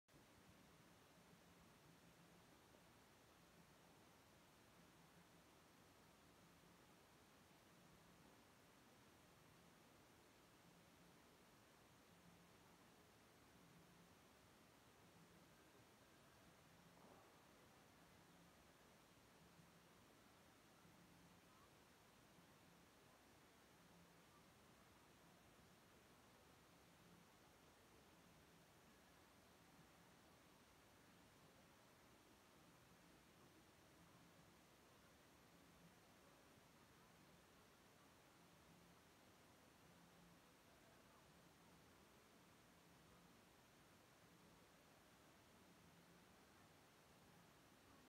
The Dubai Fountain
The Dubai Fountain is the world's largest choreographed fountain system. It is set on the 30-acre manmade Burj Khalifa Lake, at the center of the Downtown Dubai development in Dubai, United Arab Emirates.